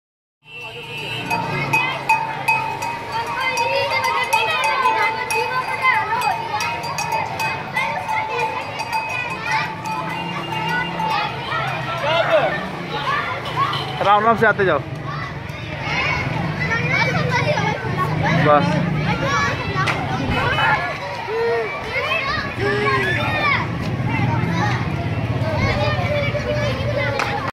Camel Ride in Lahore Streets
Kids are enjoying Camel ride first time.Luckily we found this camel man at Eid day in Abbot Road Area,Lahore,